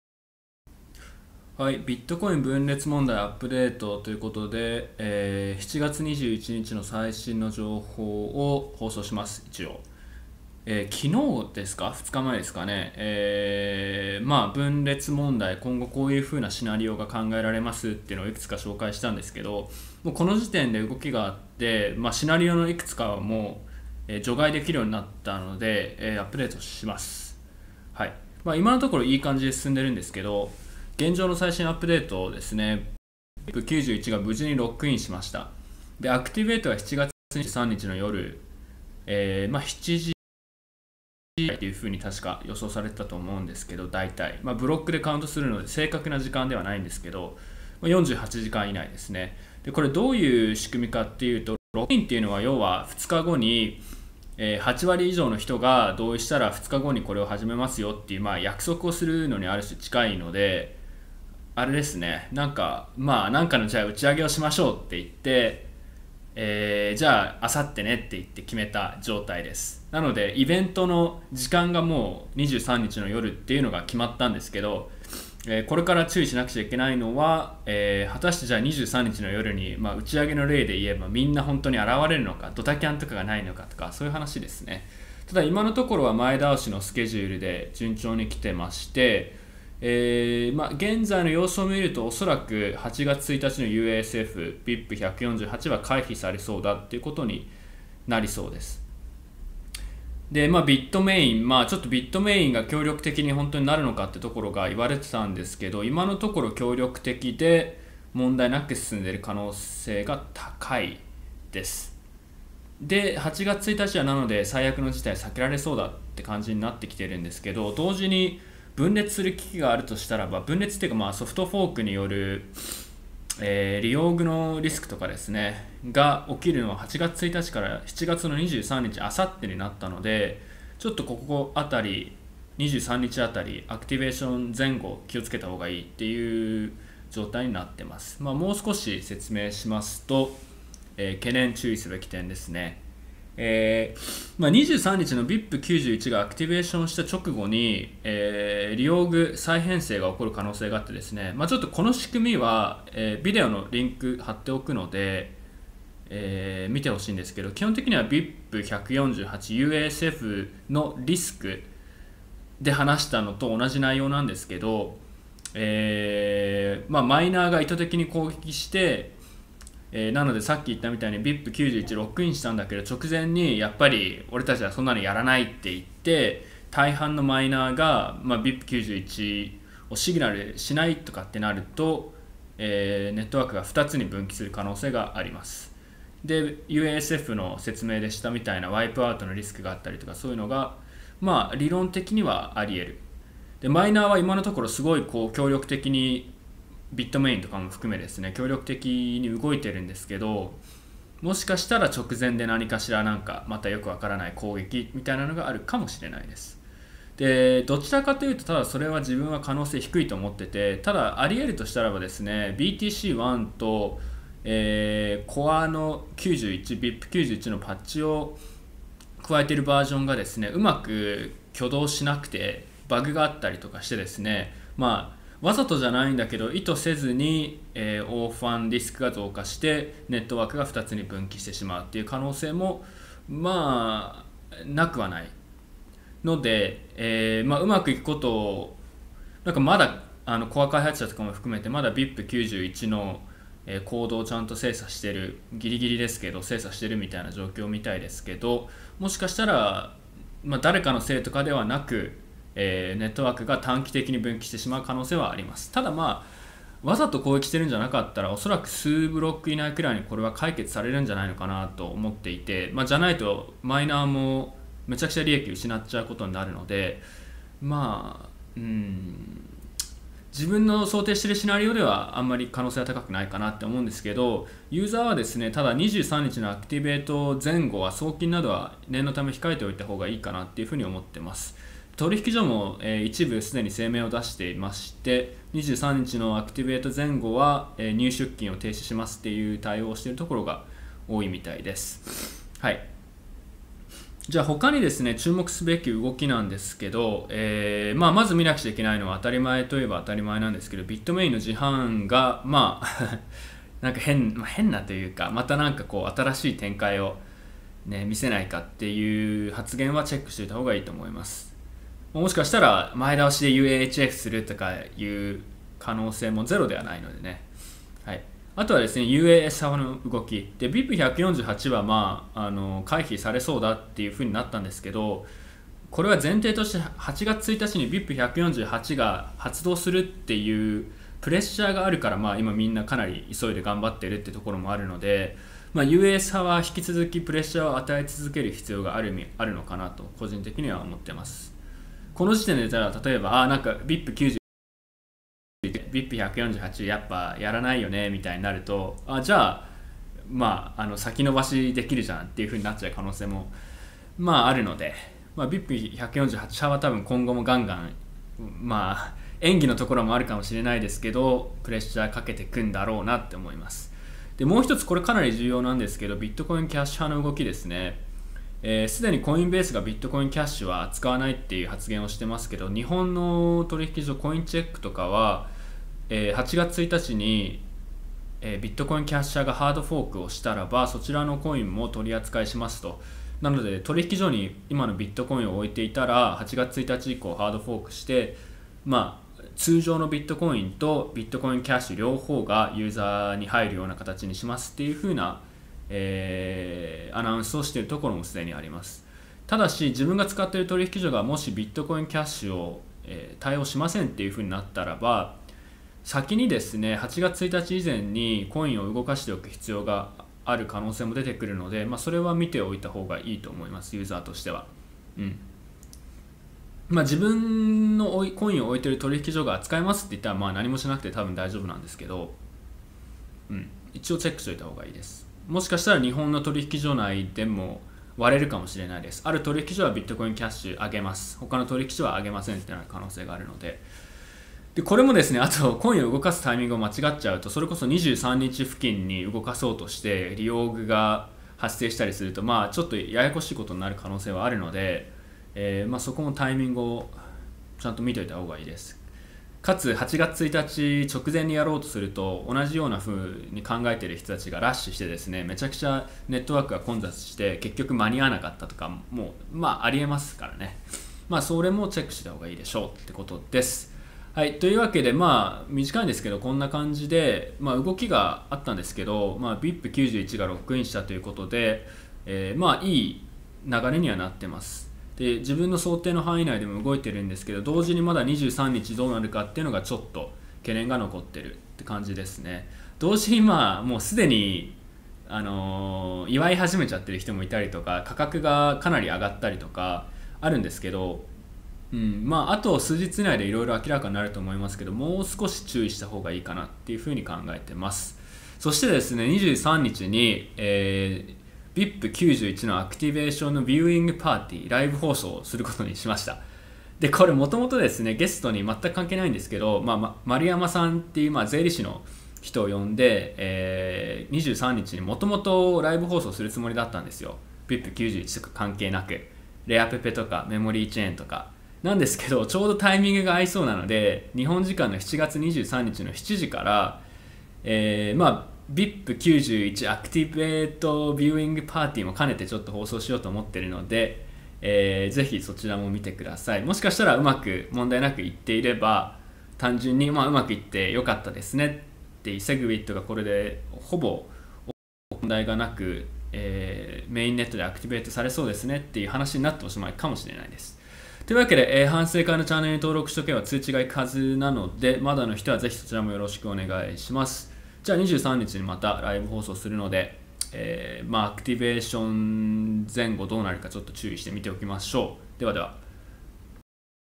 ビットコイン分裂問題/BIP91アップデート（7/21）
前回のビデオから状況に動きがあり、BIP91が無事ロックインしました。23日に実際にアクティベートされるのですが、その前後で気を付けること、注意すべき動きなどを簡単にまとめました。また、7/23の7時PMからライブ放送で、BIP91アクティベートのビューイングパーティもします。

詳しくは以下の記事も参考にしてください。